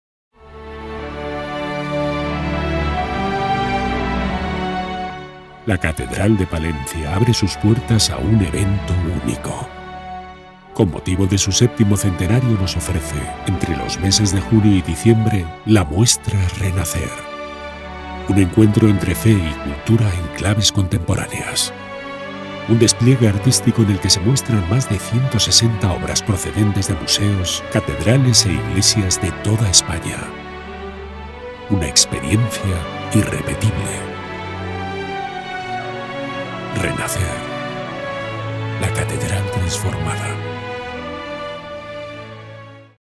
Catedral de Palencia | Es el momento perfecto para visitar RENACER
El otoño es el momento perfecto para visitar Palencia. La Catedral se ha transformado con RENACER para proporcionarte una experiencia inolvidable. Hemos construido un espacio en el que la belleza, el silencio, la historia y las emociones cobran vida para alimentar tu espÍritu.

Organiza tu visita desde nuestra web y prepara tu ánimo para disfrutar de  algo completamente diferente... Te esperamos.